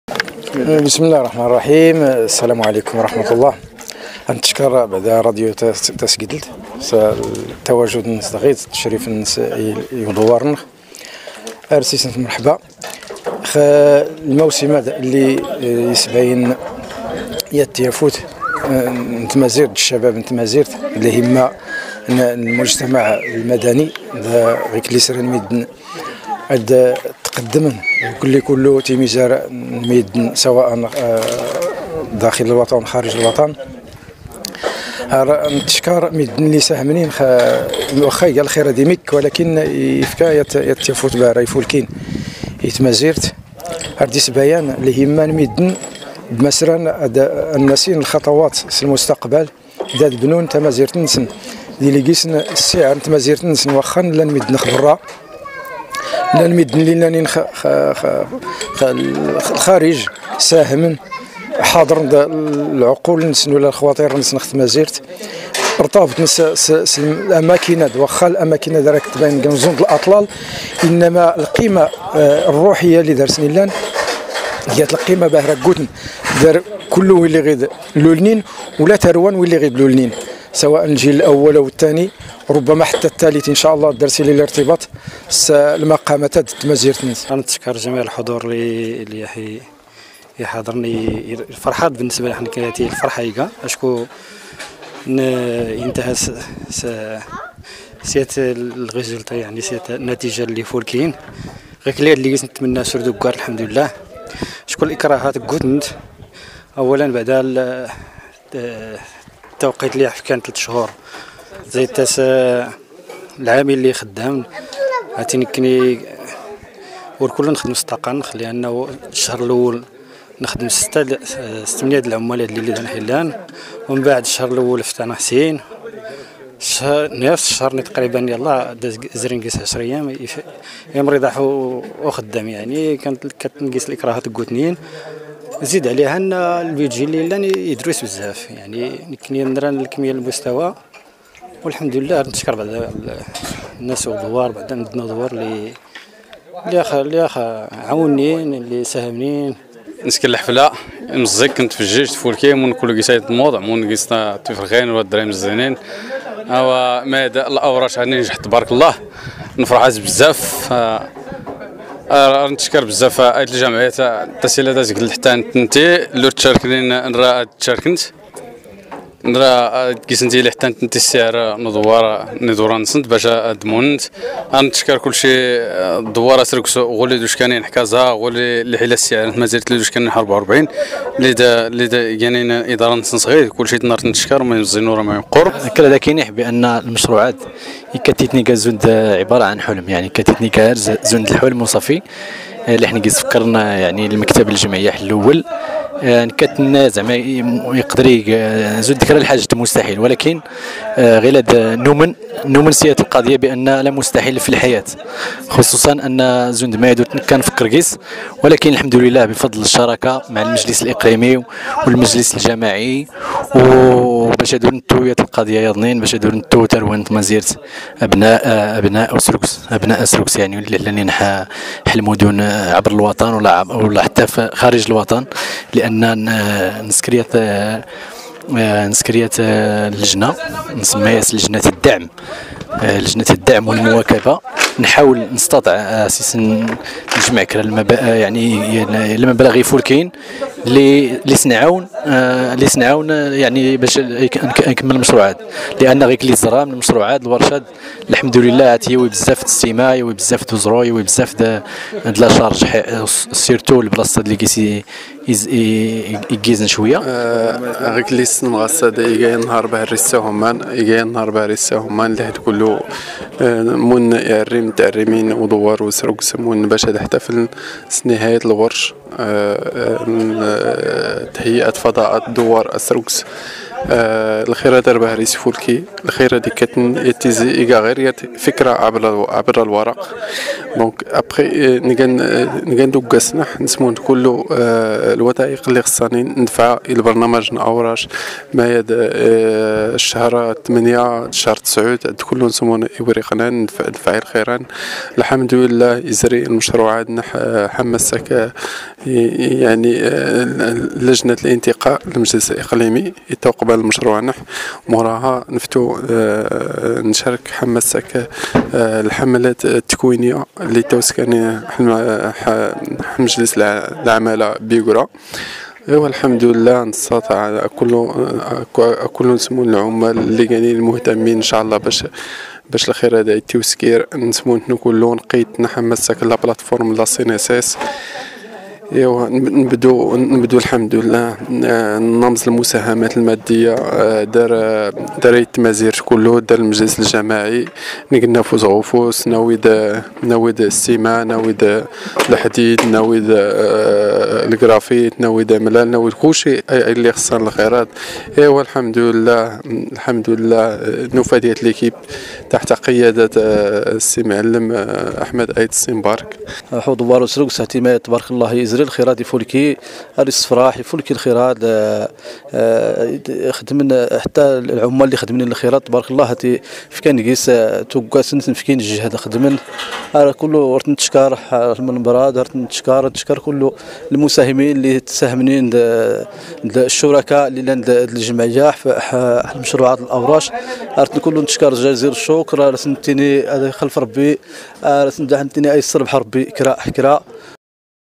تصريحات حول تدشين مشروع اوراش دوار اسركس جماعة تسكدلت
تصريحات حول تدشين مشروع دوار اسركس جماعة تسكدلت